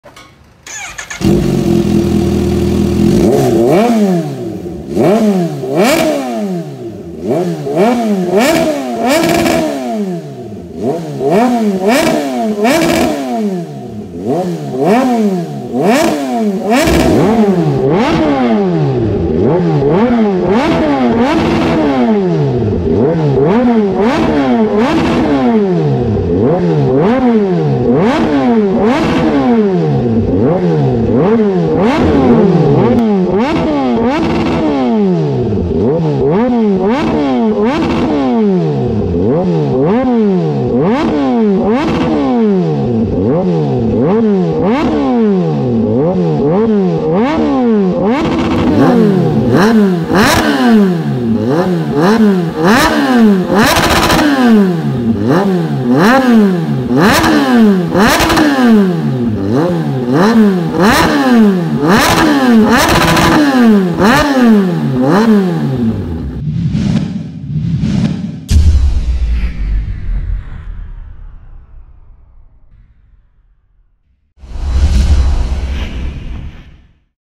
Slip-on exhaust system on a 2023 Kawasaki ZH2
DRIFT XAUST on a 2023 Kawasaki ZH2!

• Slip-on exhaust system with black knight tip